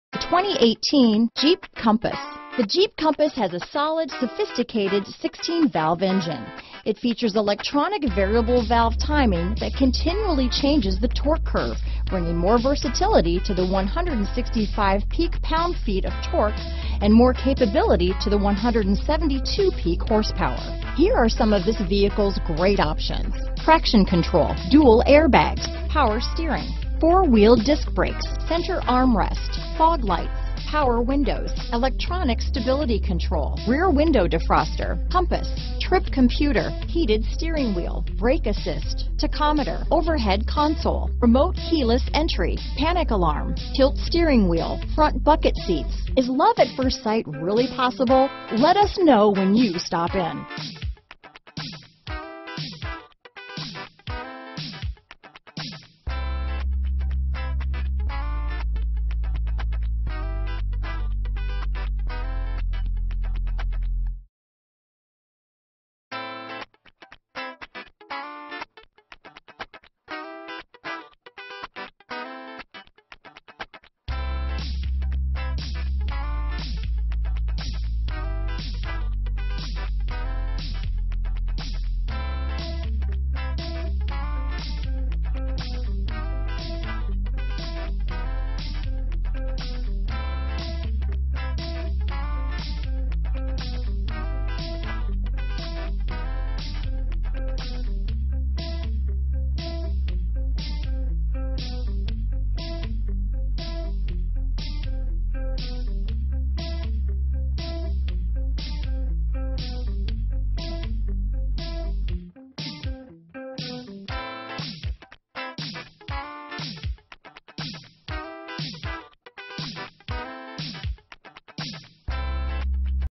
2018 Jeep Compass Owosso, East Lansing, Haslett, DeWitt, St. Johns, OK N8045
N 2018 Jeep Compass available at Slingerland Chrysler in Owosso, Michigan. Servicing the East Lansing, Haslett, DeWitt, St. Johns, MI area.

Slingerland Chrysler
3640 E M 21

2018 Jeep Compass Latitude 4WD. 30/22 Highway/City MPG